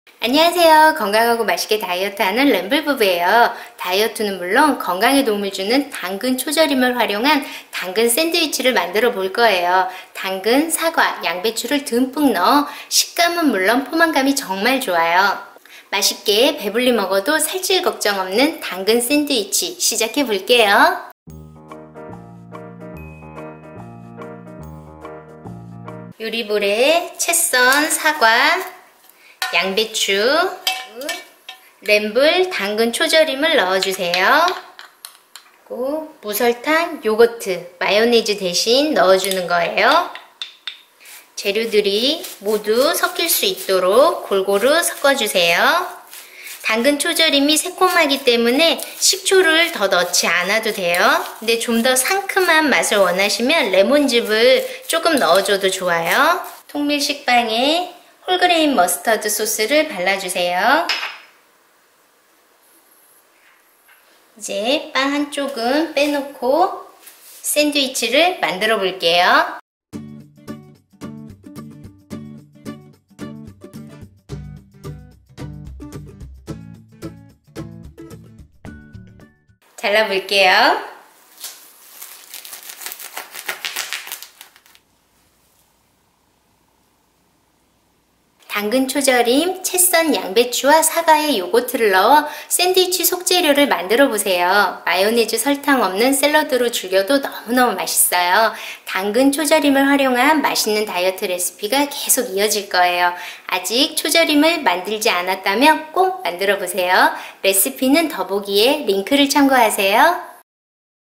초간단! 배불리 먹는 다이어트 샌드위치만들기! 램블부부 양배추샐러드 다이어트 레시피
♥ 요리재료 (1인분, 1T:15ml)
통밀식빵 2장, 램블 당근초절임 70g, 양배추 70g, 사과 100g, 슬라이스 고다치즈 2장, 청상추 6장, 무설탕요거트 2T, 홀그레인머스타드 1T

♥ 자세한 레시피는 아래 영상순서를 클릭하세요!
00:00 인트로
00:22 요리재료
00:27 양배추샐러드 만들기
00:47 샌드위치 만들기
01:29 마무리 및 정리

♥ 채널을 구독해 건강하고 맛있는 레시피를 꼭 받아보세요!